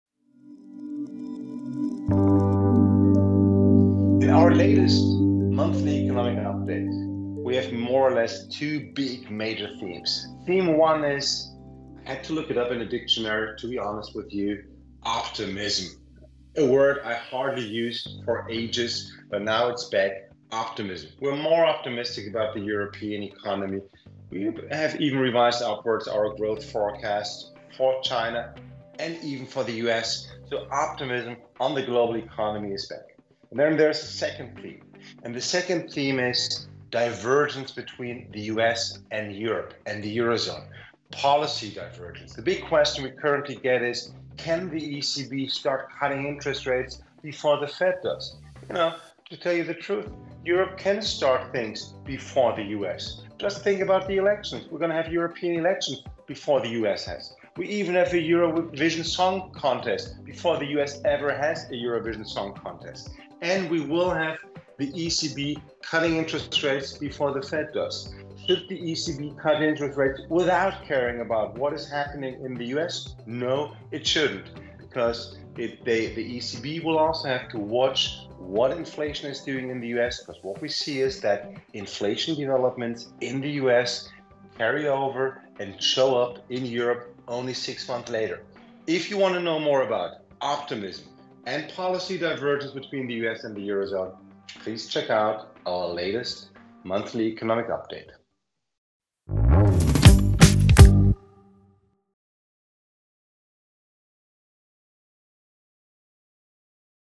Why Europe is dancing to a different tune
Optimism and divergence—that's what ING's Carsten Brzeski is talking about this month. Better times are coming for the global economy. While the US Fed hesitates about cutting interest rates, Europe's central bank is making its own kind of music and is set to make the first move next month.